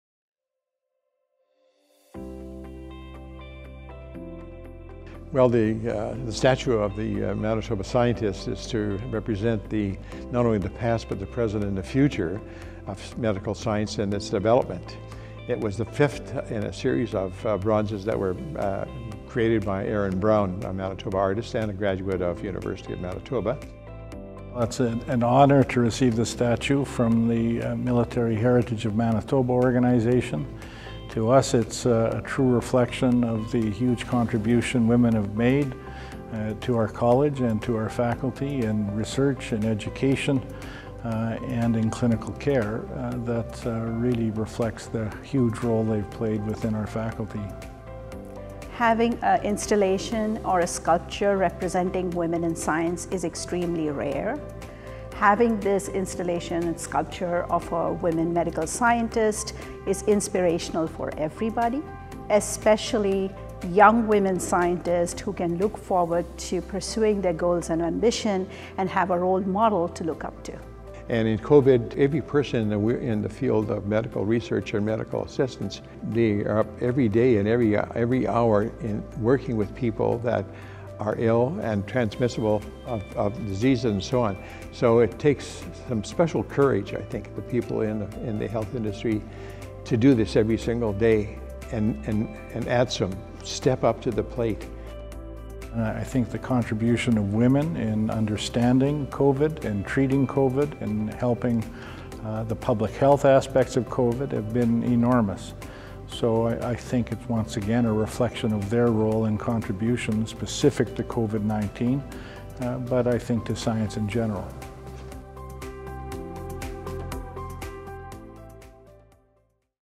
A Manitoba Scientist
A bronze sculpture has been installed on the Bannatyne campus in honour of all the health-care workers who have stepped up to fight COVID-19, and in tribute to women in medical science.

The sculpture, titled A Manitoba Scientist, has been placed on permanent display in a case on the Joe Doupe Concourse in the Basic Medical Sciences Building.

The bronze was donated to the Rady Faculty of Health Sciences by Military Heritage of Manitoba. The organization commissioned it from artist Erin Brown, an alumna of the UM School of Art, as part of a series of five bronzes on the theme of Manitobans shaping history. It represents the scientific promise of the 21st century.